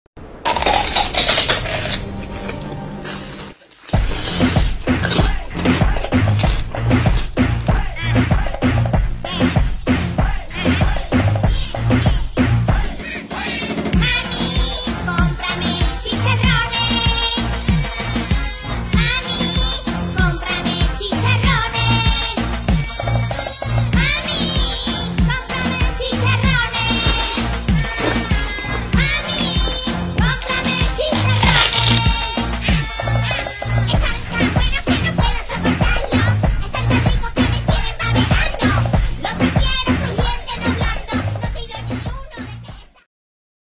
PERIJA Y SUS CHICHARRONES
Una de las vaiedades gastronómicas de mayor tendencia en el último siglo ha sido el chicharrón; una manifestación evidente de las incidencias culturales Colombianas; pero también de la búsqueda y el exquisito sabor crujiente que este alimento trae a los PERIJANEROS. Criaturas EN LA VILLA DEL ROSARIO. ! T.G. (Si te interesa pautar un comercial en nuestras redes; contáctanos